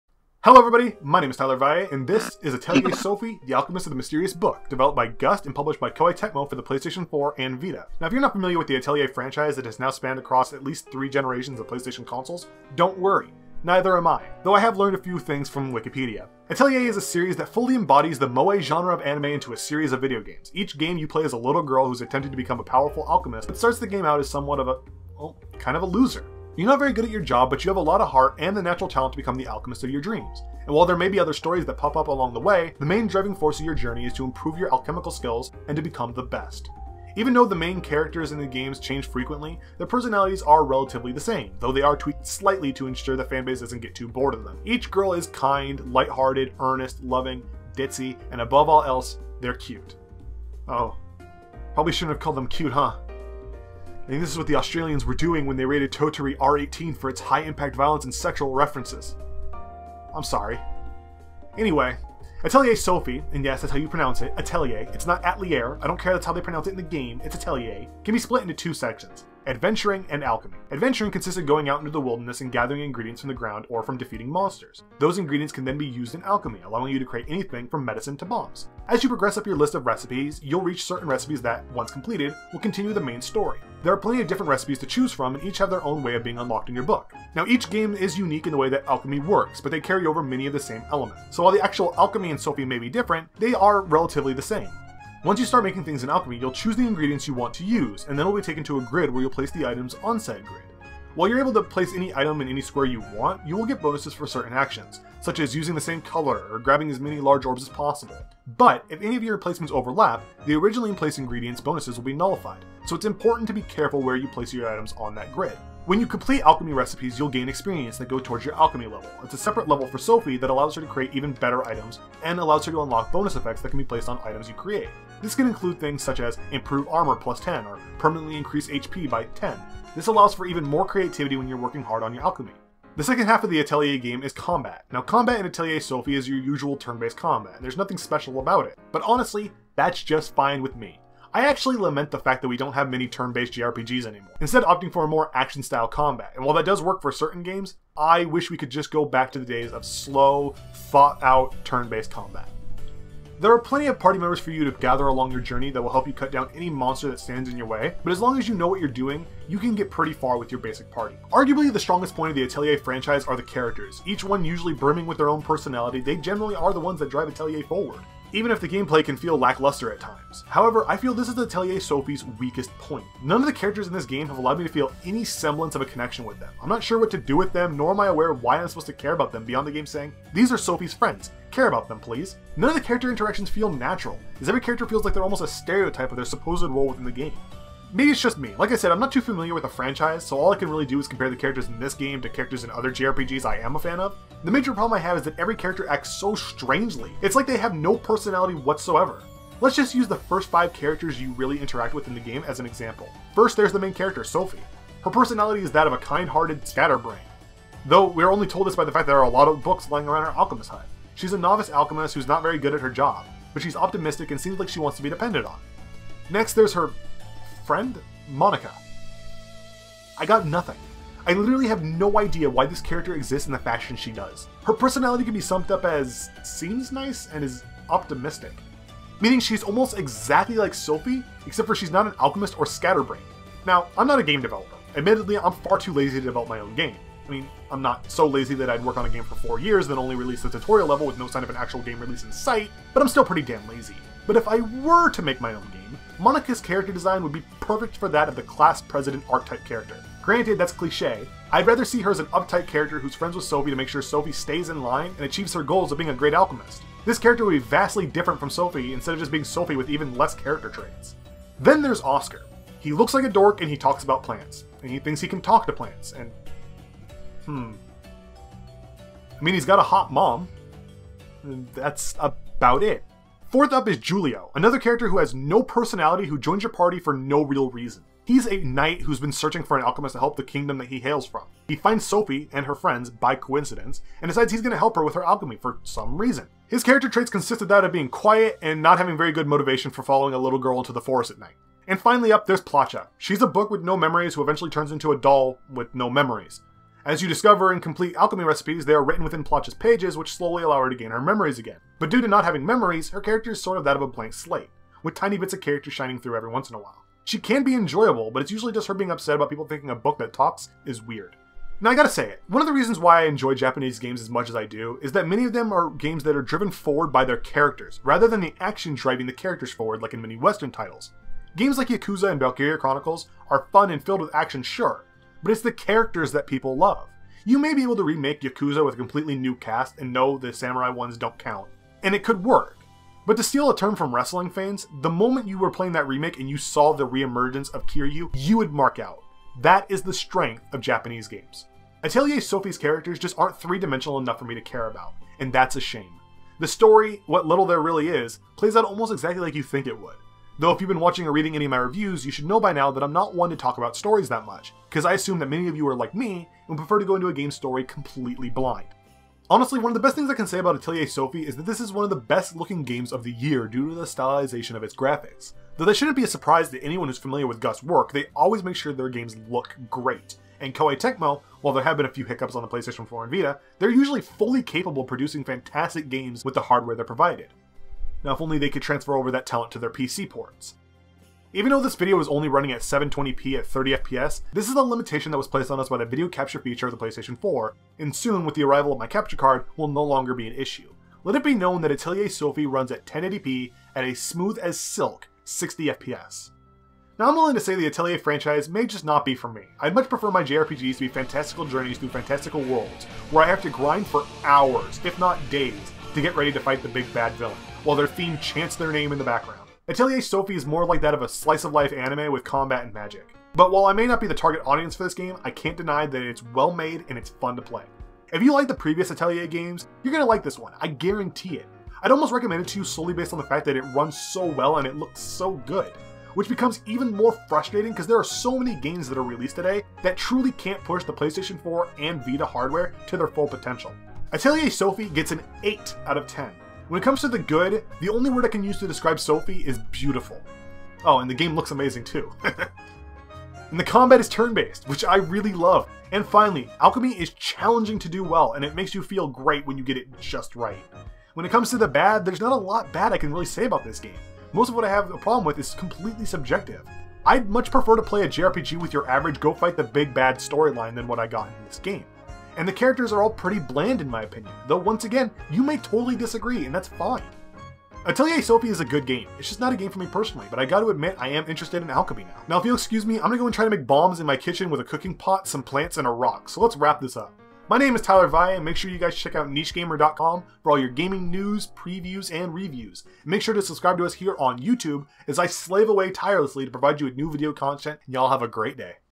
Atelier Sophie: The Alchemist of The Mysterious Book Review
Tyler finally steps away from First Person Shooters to delve into to the scary world of Atelier. Is Atelier the Alchemy simulator that you're looking for? Watch the video and find out!

*Moe anime is trash